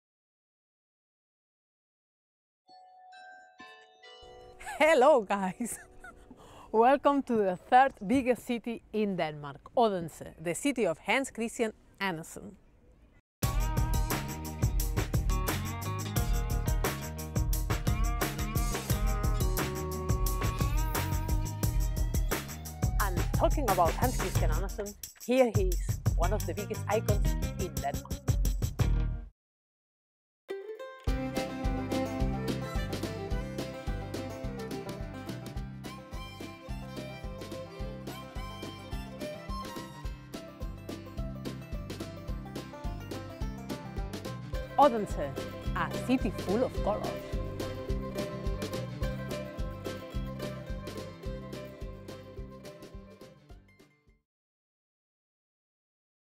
Odense, a city full of colors!
This is a sample of our work.  Our CEO visited Odense, and this is what she did there!  Take a look!